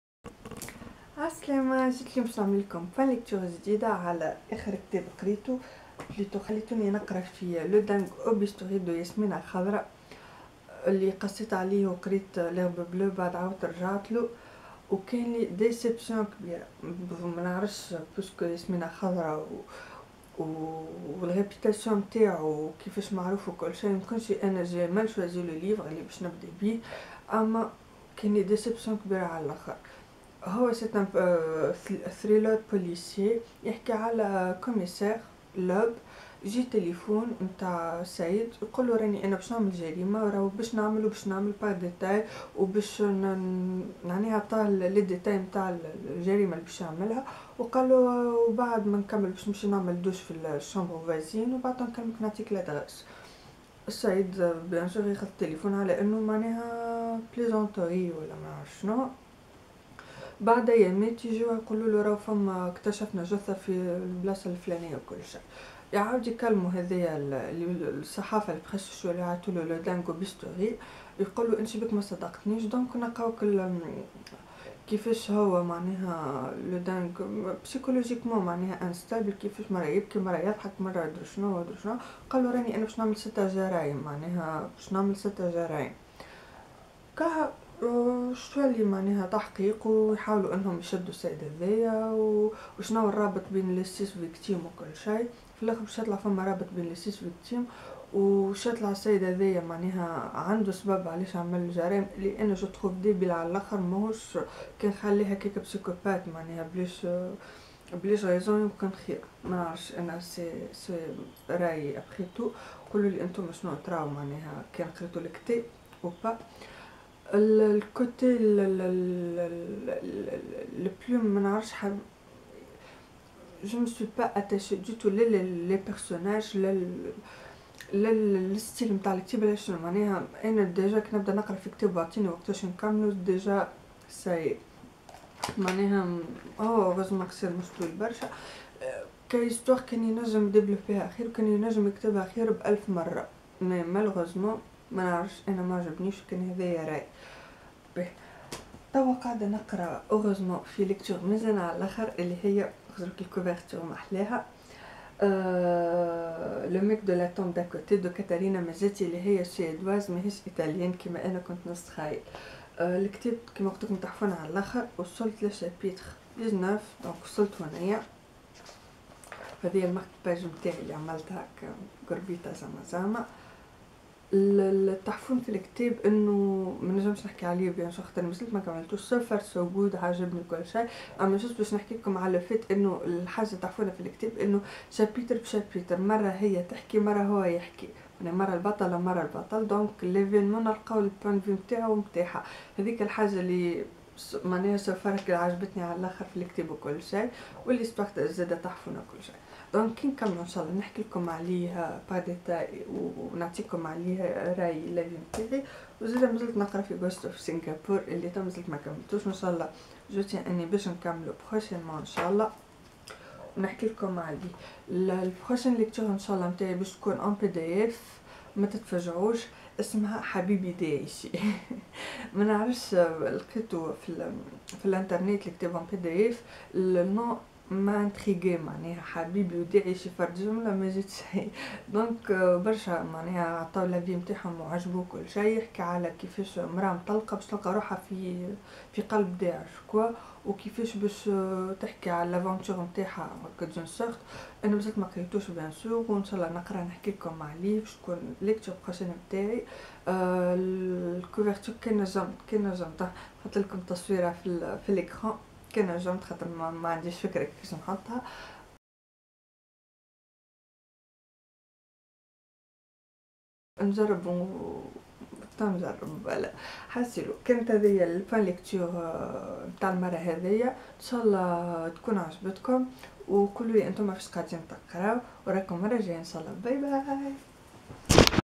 Point Lecture #11 [Le dingue au bistouri Yasmina Khadhra]
Mes lectures de cette semaine ^_^
Le dingue au bistouri: Alger en hiver. Ce jour-là, le commissaire Llob est d'humeur maussade, et le coup de téléphone qu'il reçoit n'arrange rien. Un mystérieux correspondant le prévient qu'il va commettre un crime. Le Dingue au bistouri, comme la presse ne tarde pas à le surnommer, frappe ainsi les premiers coups de sa sinistre carrière de tueur en série. Est-ce un fou ? Un pervers ? Llob ne le croit pas. Chaque meurtre est froidement prémédité, minutieusement exécuté, et toujours signé. L'homme torture ses victimes, leur arrache le cœur et dépose une étoile noire sur le cadavre. Il doit encore tuer cinq personnes. Commence alors un dialogue de fous entre Llob et le Dingue, triste et tragique rejeton d'Alger la désolée.
Me Suivre:
Me contacter:
Livres Mentionnées :
Le dingue au bistouri Yasmina Khadhra 2/5
Ghosts of Singapore! Goh Sin Tub2/5
Le mec de la tombe d'à côté Katarina Mazzeti 3/5
حبيبي داعشي هاجر عبد الصمد